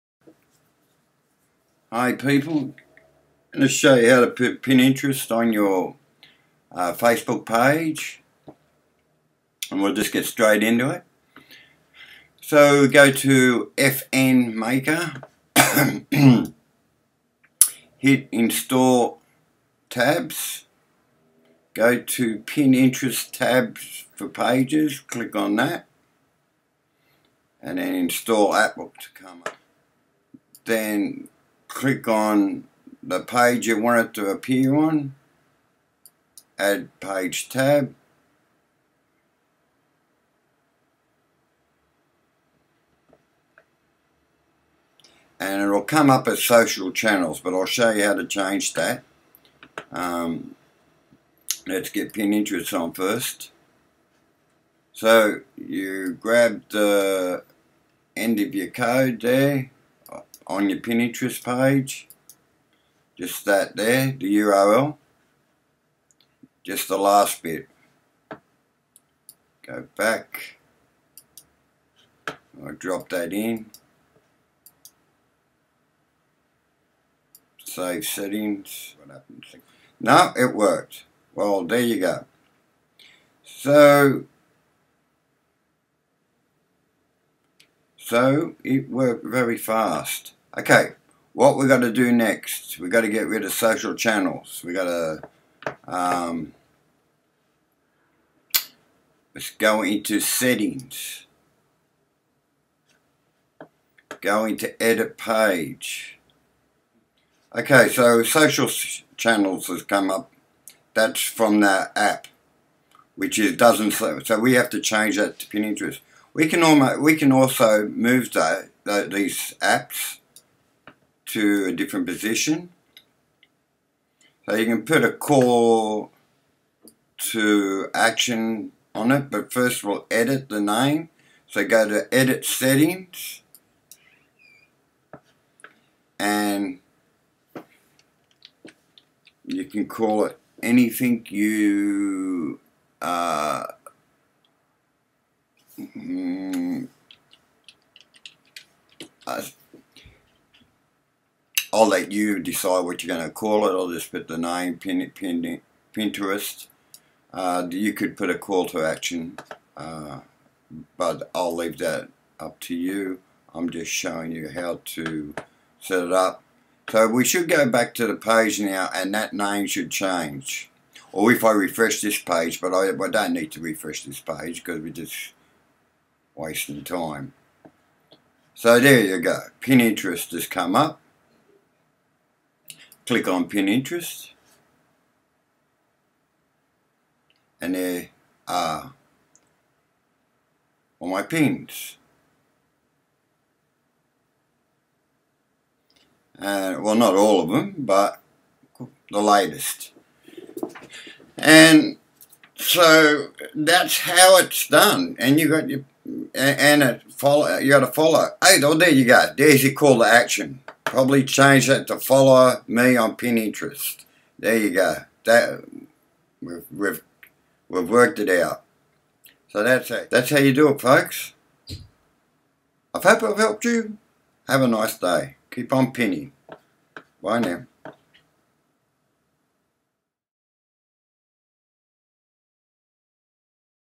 How to add Pinterest App and Board to a facebook page
How to add Pinterest Board on Facebook Page showing the latest Pins, This is the to the app you'll need to do this Project.